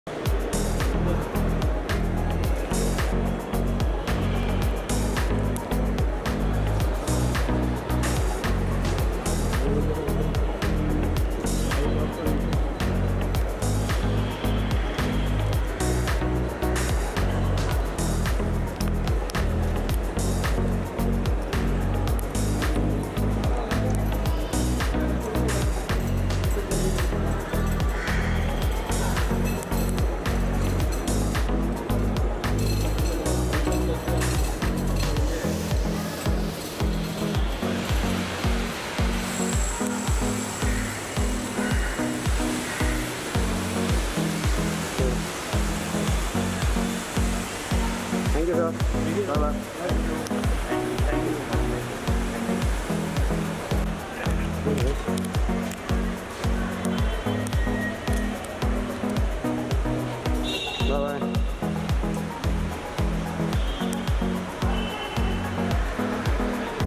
Katrina Kaif Vicky Wedding: शादी के बाद मुंबई लौटा Vicky परिवार; Watch video |FilmiBeat
Katrina Kaif Vicky Kaushal Wedding, Vicky's family arrive back to Mumbai after wedding.  Katrina & Vicky's Saat Phere are complete at Six Senses Fort. Watch Out Video to know more.